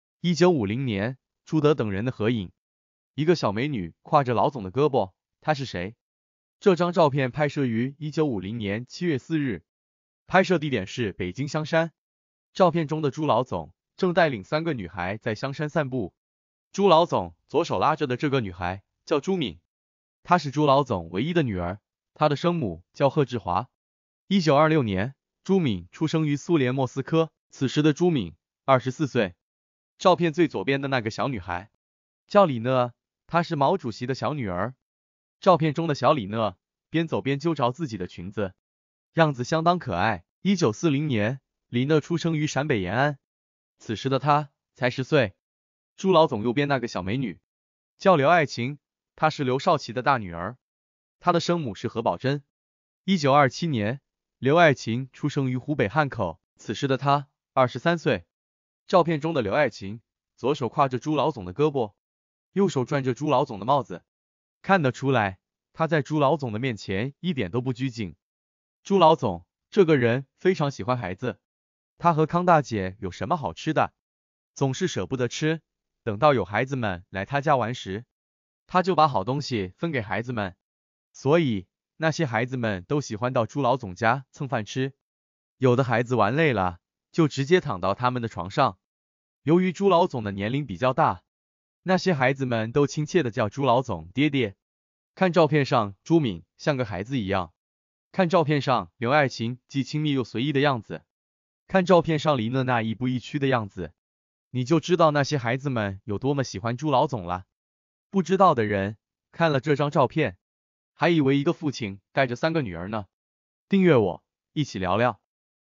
1950年，朱德等人的合影，一个小美女挎着老总的胳膊，她是谁？ In 1950, in the group photo of Zhu De and others, a little beauty is
In 1950, in the group photo of Zhu De and others, a little beauty is holding the boss's arm. Who is she?
1950年，朱德等人的合影，一个小美女挎着老总的胳膊，她是谁？